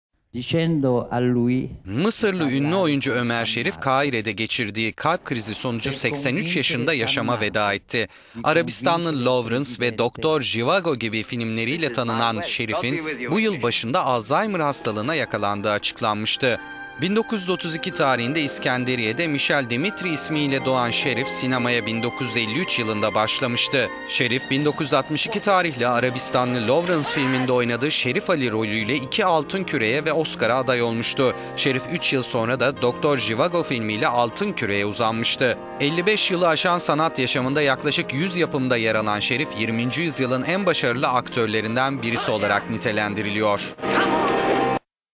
Mısırlı ünlü aktör Ömer Şerif yaşama veda etti
Mısırlı ünlü oyuncu Ömer Şerif Kahire'de geçirdiği kalp krizi sonucu 83 yaşına yaşama veda etti.

*Arabistanlı Lawrence* ve *Dr. Jivago* filmleriyle tanınan Şerif'in bu yıl başında Alzheimer hastalığına yakalandığı açıklanmıştı.

1932 tarihinde İskenderiye'de Michel Demitri Chalhoub ismiyle doğan Şerif sinemaya 1953 yılında başlamıştı.

Şerif, 1962 tarihli Arabistanlı Lawrence filminde oynadığı Şerif Ali rolüyle iki Altın Küre’ye ve Oscar'a aday olmuştu. Şerif üç yıl sonra da Doktor Jivago film…